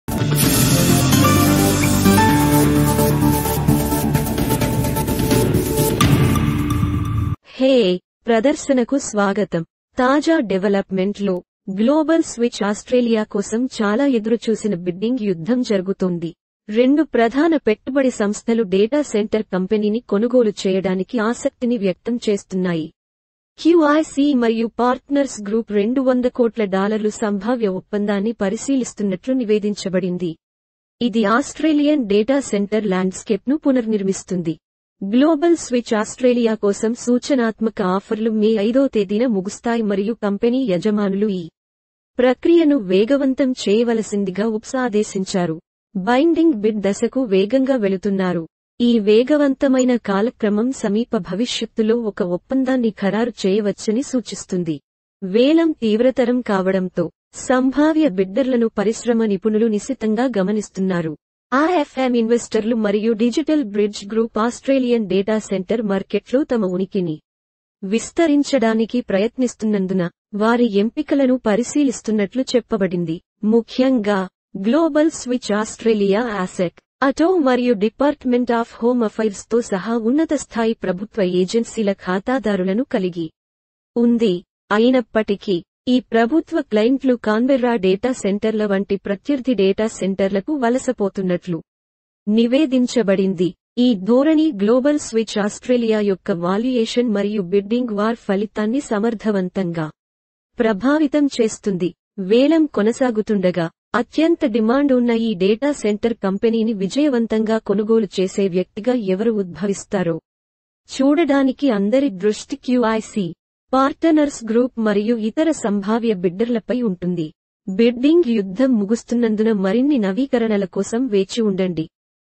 (TELUGU) వ్యాపార ప్రపంచంలో ప్రధాన సముపార్జన ఒప్పందాలు
ఎక్వమ్ క్యాపిటల్ ఇటీవల రెండు-దశల సీనియర్ టర్మ్ లోన్ సదుపాయాన్ని పూర్తి చేసి $6MM వెంచర్ రుణాన్ని ఎగ్జిట్ రుసుములతో పాటు అదనంగా రీఫైనాన్స్ చేసింది. సూచనాత్మక ఆఫర్‌లు మే 5న అందజేయబడతాయి మరియు వేలం బైండింగ్ బిడ్ దశను వేగంగా ట్రాక్ చేయమని గ్లోబల్ స్విచ్ యజమానులు UBSని ఆదేశించారు.